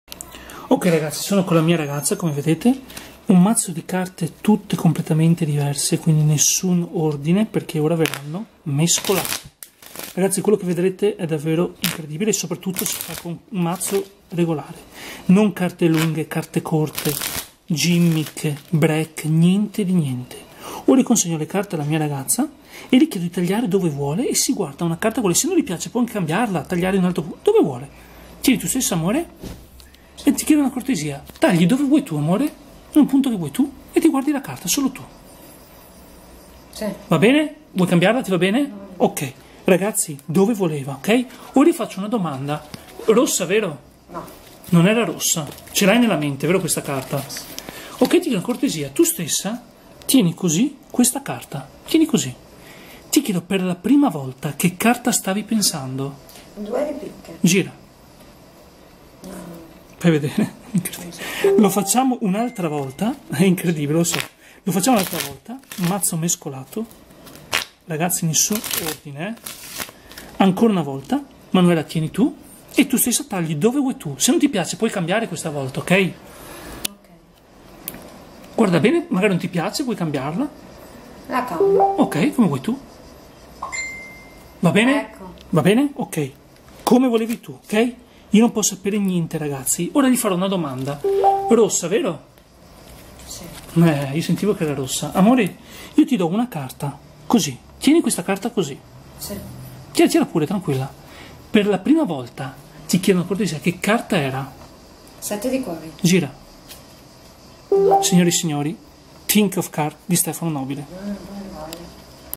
SEXIEST CARD TRICK EVER 2016!!!(My Girl WET?)
INFORMAZIONI INVISIBILI 2017(THINK A CARD) di Stefano Nobile
si basa su informazioni invisibili..
solo una domanda..da un mazzo mescolato...
grazie amici.
S.N.